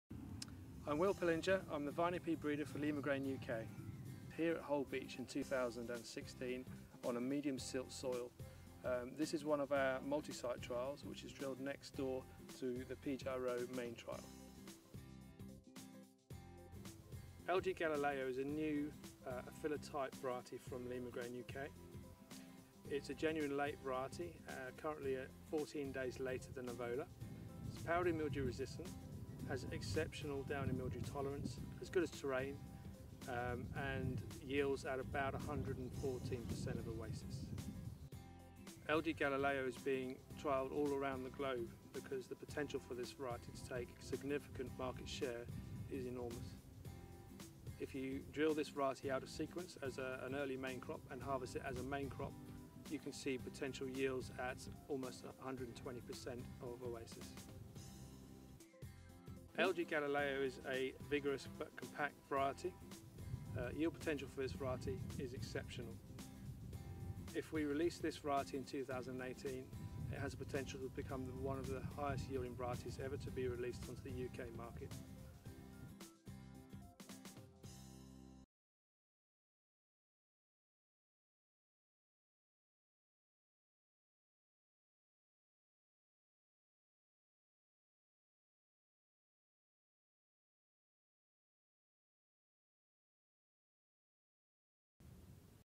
1 Min Variety Profile - LG Galileo Vining Peas
Will Pillinger, Vining Pea Breeder for Limagrain UK introduces the new vining pea variety; LG Galileo.

Limagrain introduces its new rape/kale hybrid variety Unicorn